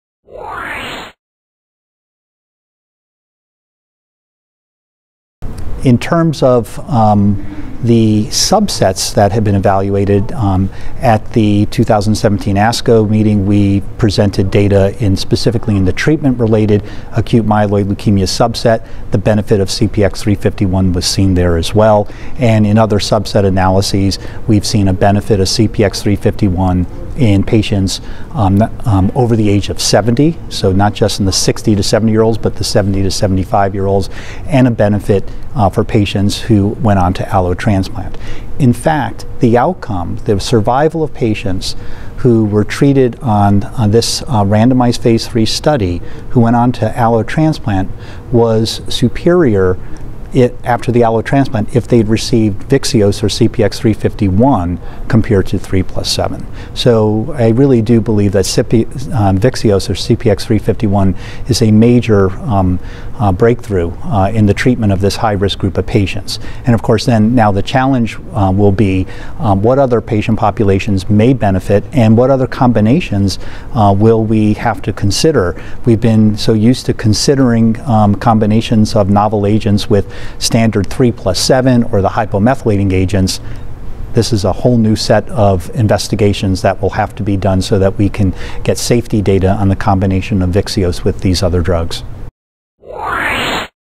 Dr. Erba Discusses Efficacy of CPX-351 in Additional Subsets of AML
Harry Erba, MD, PhD, professor of medicine, director, University of Alabama (UAB) Hematologic Malignancy Program, UAB School of Medicine, discusses additional subsets of acute myeloid leukemia (AML) that CPX-351 (Vyxeos) demonstrates efficacy in.